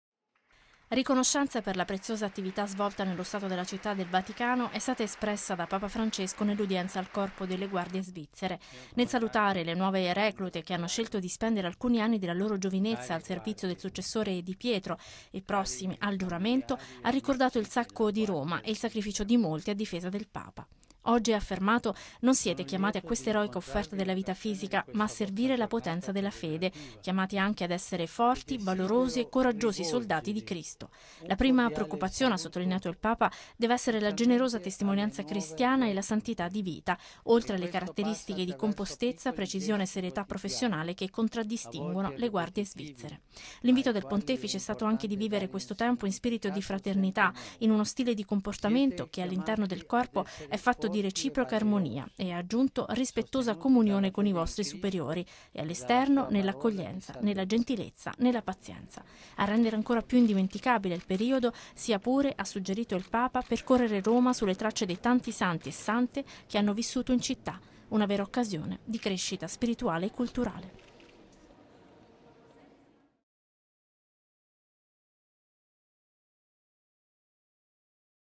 Udienza Guardie Svizzere 06-05-2017
Servite la potenza della fede

Papa Francesco ha ricevuto le nuove reclute e gli ufficiali delle Guardie Svizzere, invitandoli a crescere come coraggiosi “soldati di Cristo” con uno stile di comportamento all’insegna dell’armonia e dell’accoglienza.